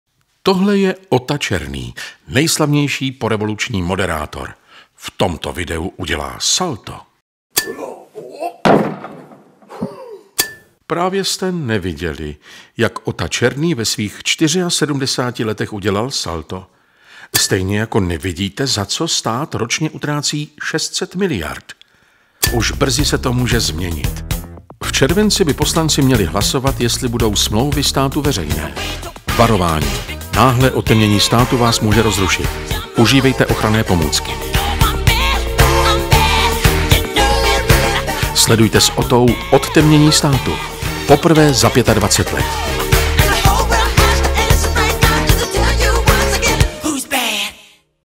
Udělá Otta Černý stále salto?
Kdy nastane na celém území České republiky odtemnění? Podle Bohuslava Sobotky do léta. Podle astronomů spíš až v roce 2349.

Zajímá vás, kolik stát utratil od začátku roku bez veřejné kontroly? Tak navštivte stránky a zjistěte aktuální číslo.